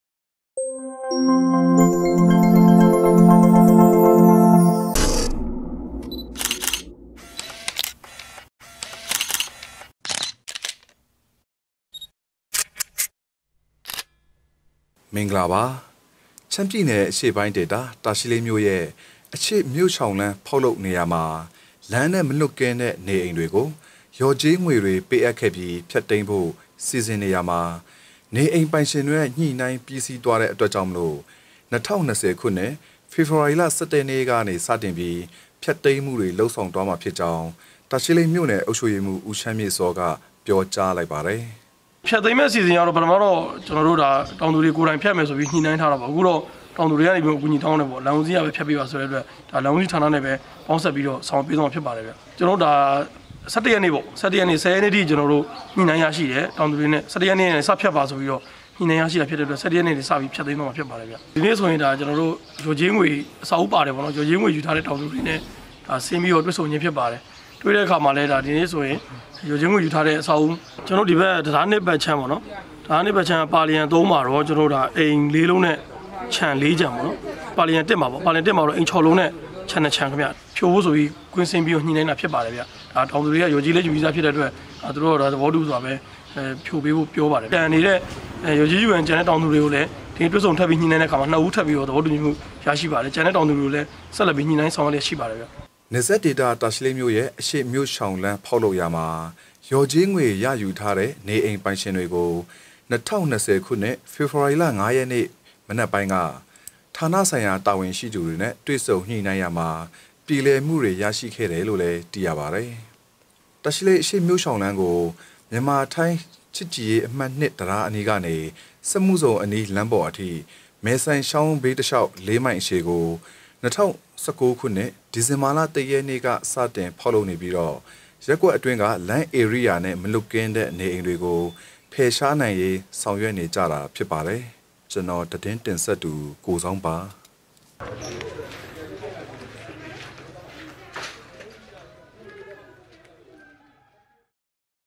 Tachileik News Agency : တာချီလိတ်မြို့ရှောင်လမ်းနှင့် မလွတ်၍ လျော်ကြေးပေးထားသည့် နေအိမ်များကို...
တာချီလိတ်မြို့ရှောင်လမ်းနှင့် မလွတ်၍ လျော်ကြေးပေးထားသည့် နေအိမ်များကို ဖေဖော်ဝါရီလ(၁၁)ရက်နေ့စဖျက်မည်

ရှမ်းပြည်နယ်အရှေ့ပိုင်း တာချီလိတ်မြို့၏ အရှေ့မြို့ရှောင်လမ်း ဖောက်လုပ်နေရာမှာ လမ်းနဲ့ မလွတ်ကင်းတဲ့ နေအိမ်တွေကို လျော်ကြေးငွေတွေ ပေးအပ်ခဲ့ပြီး ဖျက်သိမ်းဖို့ စီစဉ်နေရာမှာ နေအိမ်ပိုင်ရှင်တွေနဲ့ညှိနိုင်းပြီးစီးသွားတဲ့အတွက် ၂၀၂၀ ခုနှစ် ဖေဖော်ဝါရီလ (၁၁) ရက်နေ့ကနေ စတင်ပြီး ဖျက်သိမ်းမှုတွေလုပ်ဆောင်သွားမှာ ဖြစ်ကြောင်း တာချီလိတ်မြို့နယ်အုပ်ချုပ်ရေးမှူး ဦးချမ်းမြေ့စောက ပြောပါတယ်။

တာချီလိတ်မြို့ အရှေ့မြို့ရှောင်လမ်းဖောက်လုပ်ရာမှာ လျော်ကြေးငွေရယူထားတဲ့ နေအိမ်ပိုင်ရှင်တွေကို ၂၀၂၀ ခုနှစ် ဖေဖော်ဝါရီလ (၅) ရက်နေ့ မနက်ပိုင်းက ဌာနဆိုင်ရာတာဝန်ရှိသူတွေနဲ့ တွေဆုံညှိနှိုင်းရာမှာ ပြေလည်မှုရရှိခဲ့တာဖြစ်ပါတယ်။

တာချီလိတ်မြို့ရှောင်လမ်းကို မြန်မာ-ထိုင်း ချစ်ကြည်ရေးအမှတ်(၂)တံတားအနီးကနေ စက်မှုဇုန်အနီး လမ်းပေါ်အထိ မယ်ဆိုင်ချောင်းတစ်လျှောက် (၄) မိုင်အရှည်ကို ၂၀၁၉ ခုနှစ် ဒီဇင်ဘာလ (၁) ရက်နေ့ကစတင်ဖောက်လုပ်နေပြီးတော့ ရပ်ကွက်အတွင်းက လမ်းဧရိယာနဲ့မလွတ်ကင်းတဲ့အိမ်တွေကို ဖယ်ရှားနိုင်ရေး ဆောင်ရွက်နေပါတယ်။

တင်ဆက် - ကိုဇောင်း

သတင်းရိုက်ကူး - ဟန်စိုးထိုက်